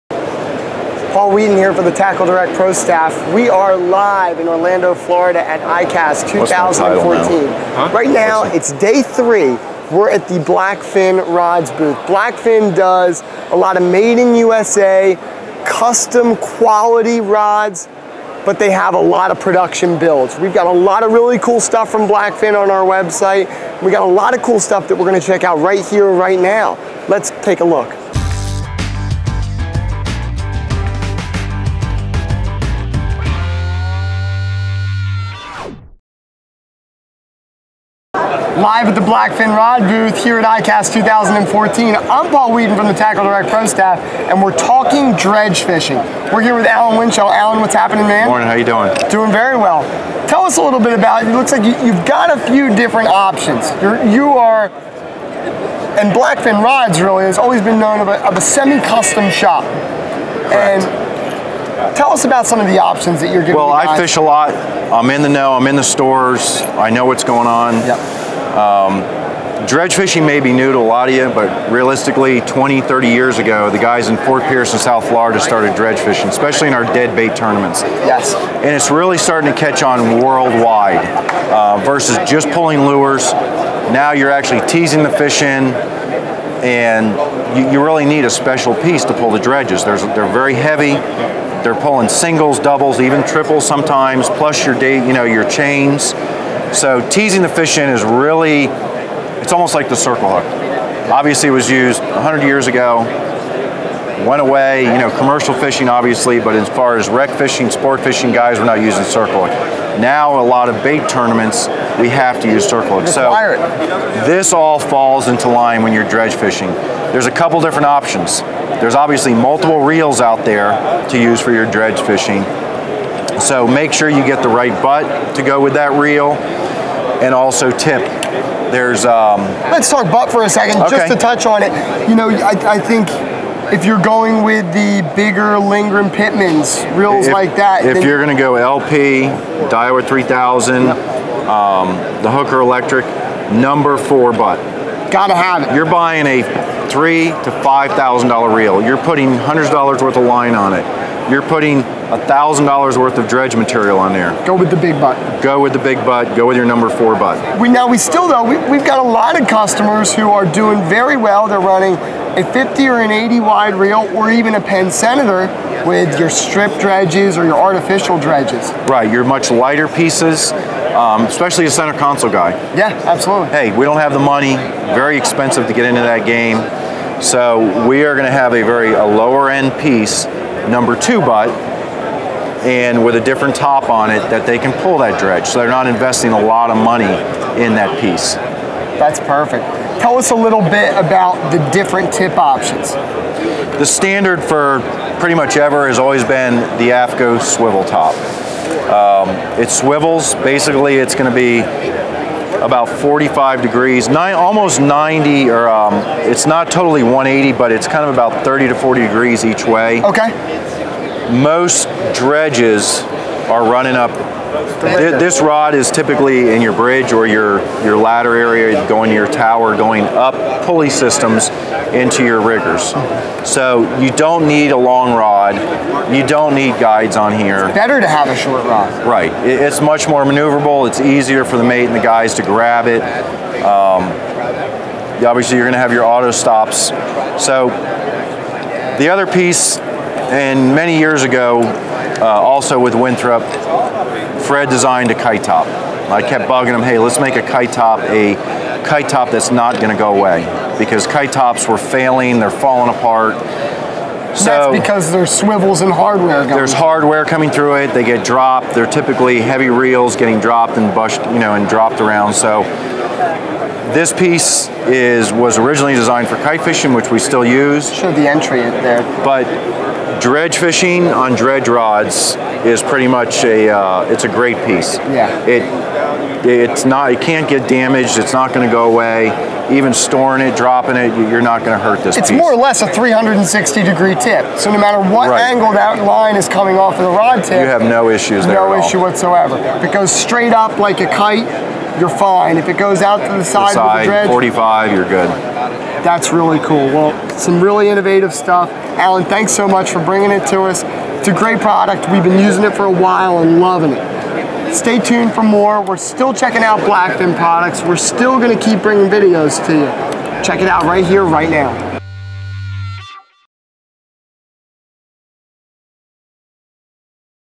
Blackfin Teaser Rod at ICAST 2014
Paul Wheaton from the TackleDirect Pro Staff and Allen Winchel from Blackfin discuss the features of the Blackfin Teaser Rod at ICAST 2014 in Orlando, Florida.